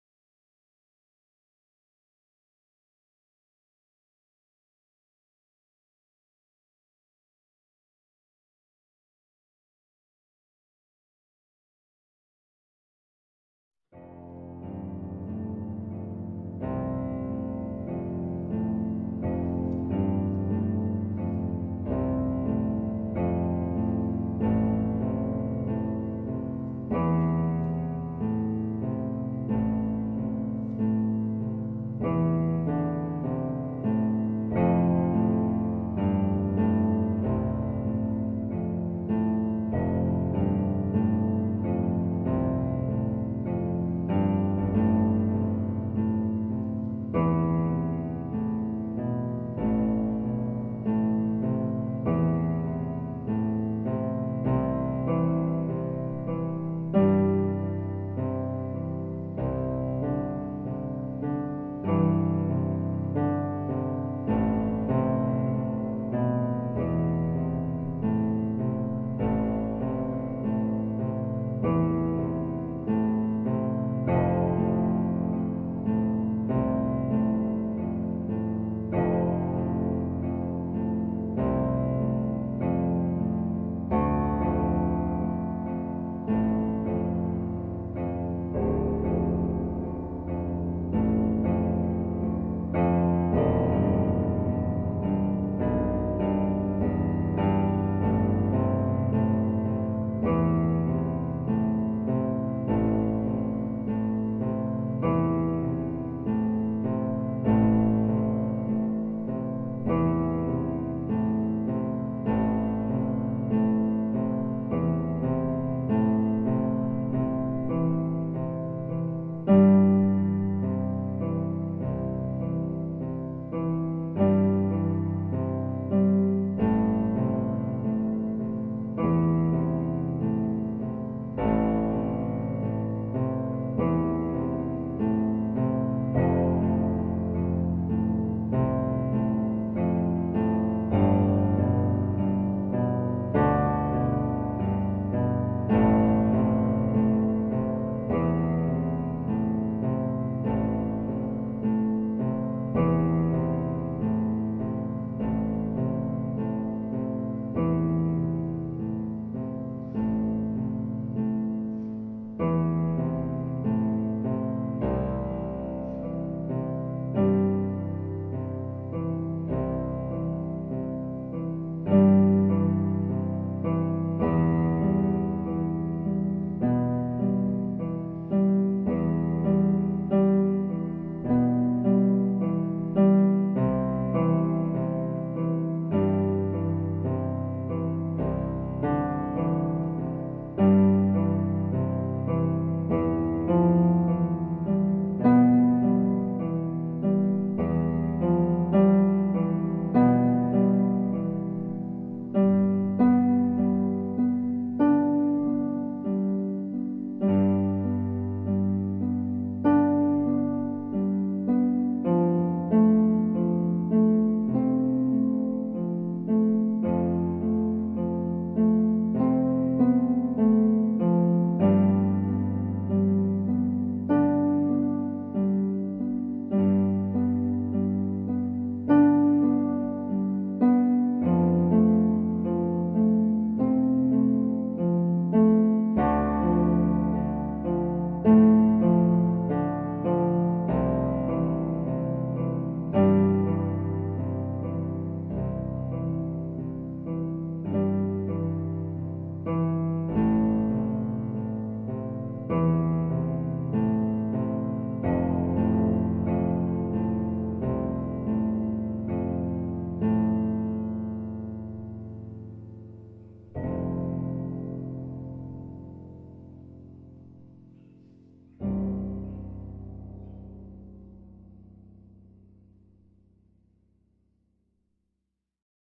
The Prayer for Divine Love.
The only prayer we need offer to our father for her Divine love and truth to come into the human soul who sincerely asks for it.